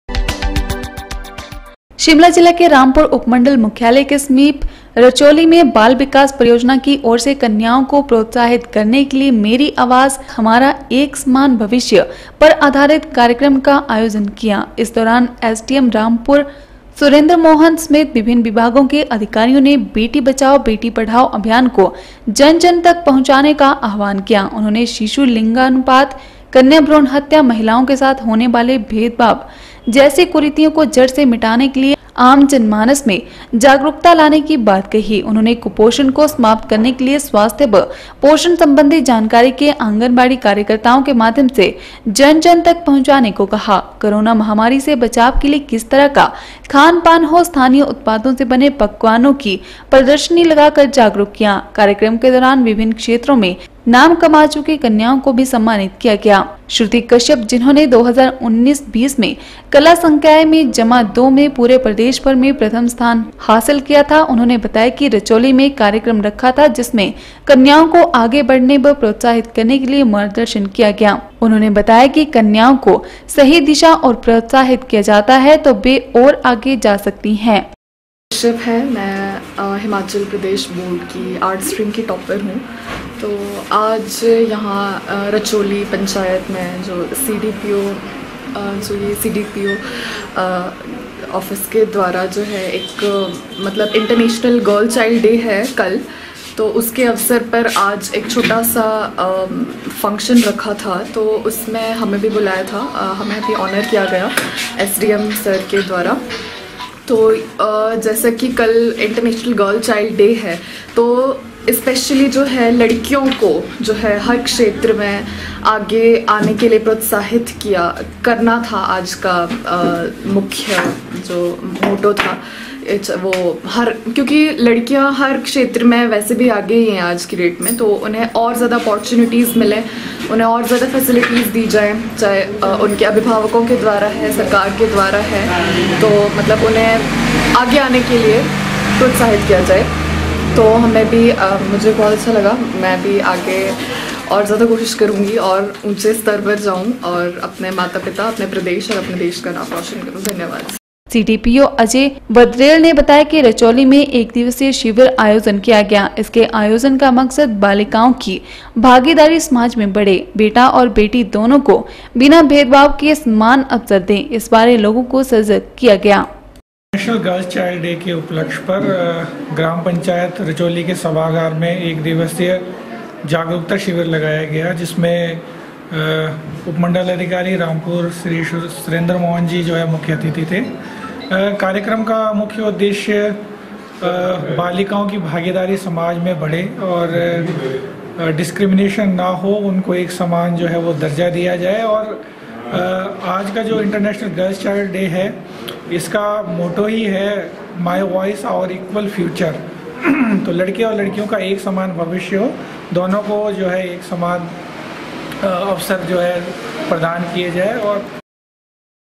14 OCT 12 विभिन्न क्षेत्रों में नाम कमा चुकी कन्याओ को आईसीडीएस दवारा कियासम्मानित।
मेरी आवाज हमारा एक समान भविष्य” थीम के साथ रामपुर के रचोली में
विभिन्न क्षेत्रों में नाम कमा चुकी कन्याओ को आईसीडीएस दवारा  किया
सम्मानित।   बेटियों की समाज निर्माण में  क्यों है अहम भूमिका दी गई
जानकारी।  इस दौरान स्थानीय पारम्परिक अनाज से बनाये गए पकवानो को भी
किया गया प्रदर्शित।विभिन्न विभागों के अधिकारियो ने लोगो को इस कोरोना
काल में रोग प्रतिरोधक क्षमता बढ़ाने के दिए टिप्स।